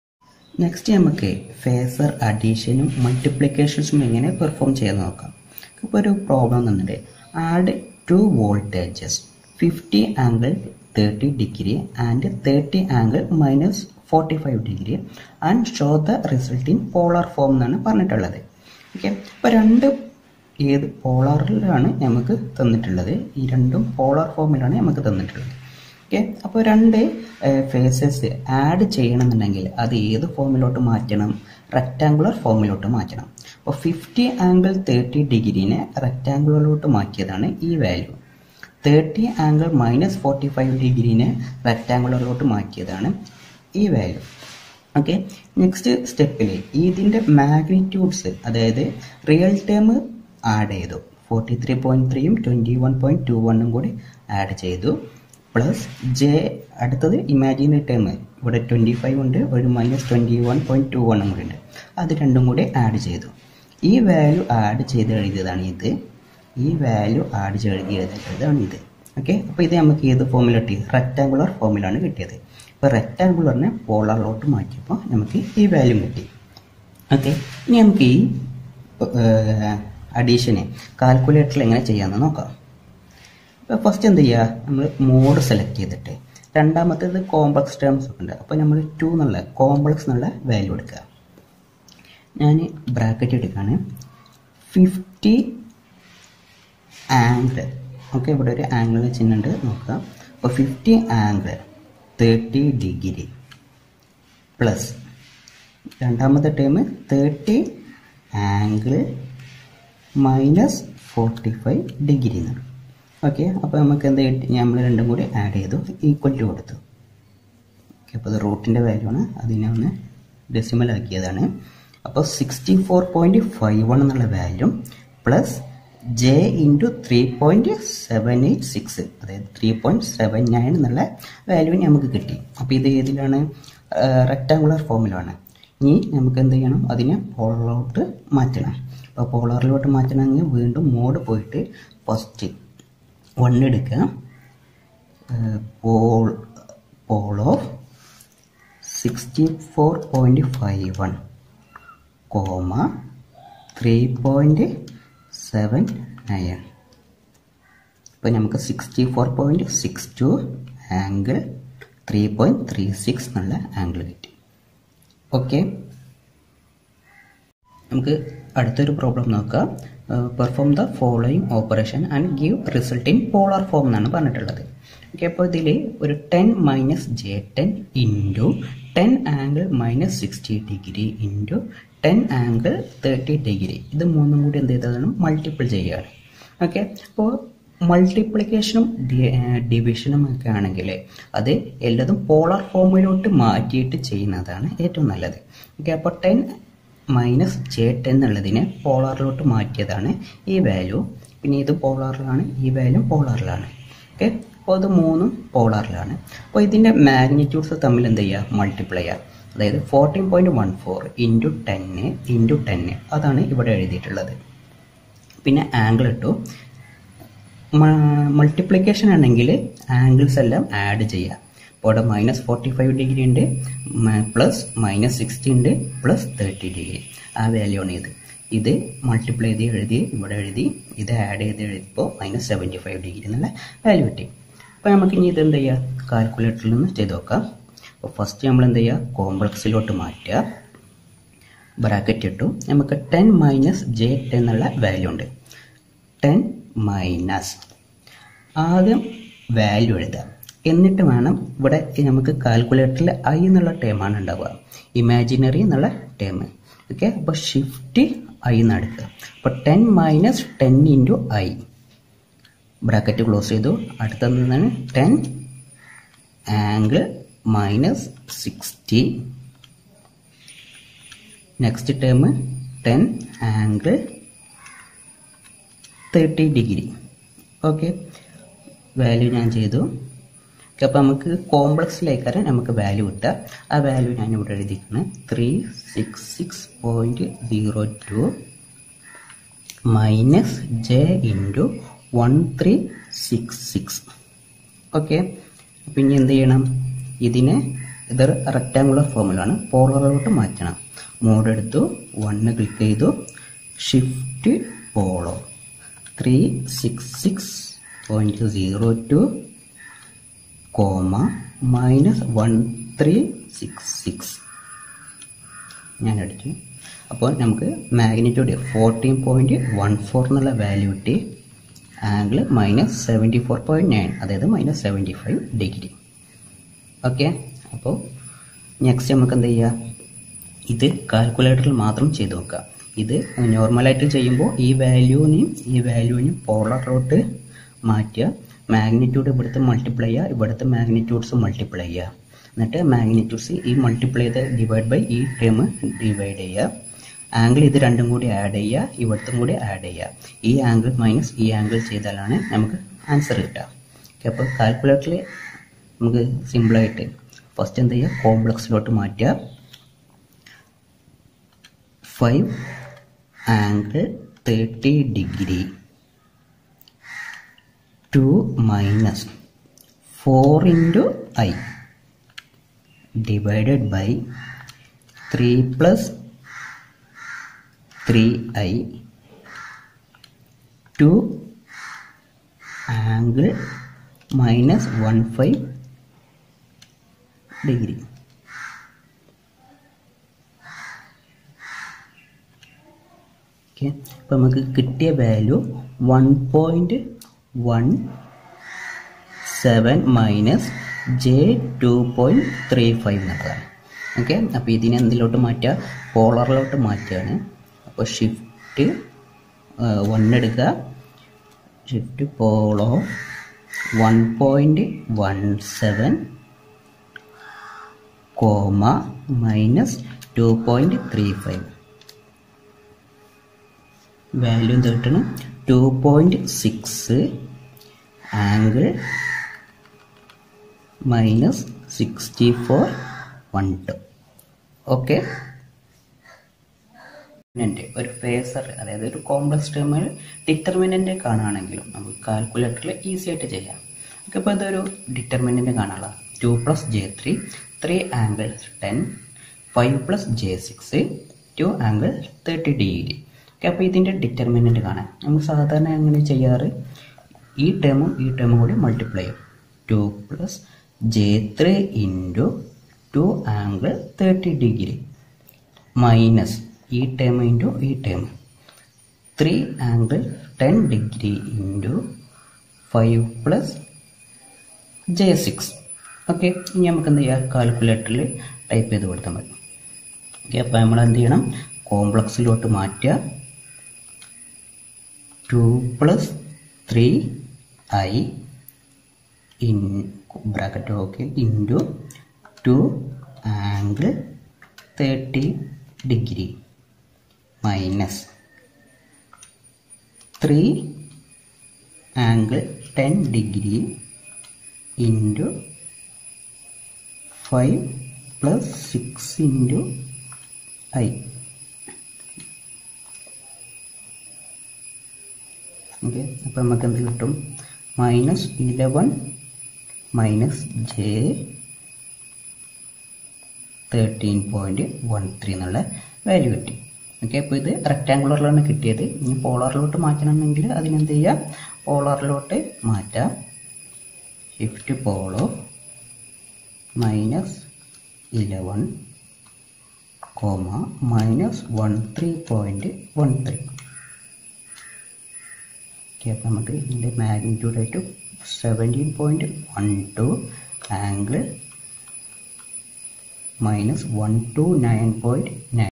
COMPLEX OPERATIONS USING CALCULATOR IN MALAYALAM- ADDITION, SUBTRACTION, MULTIPLICATION AND DIVISION
PHASOR OPETATIONS
ADDITION
SUBTRACTION
MULTIPLICATION
DIVISION
DETERMINANT OF A MATRIX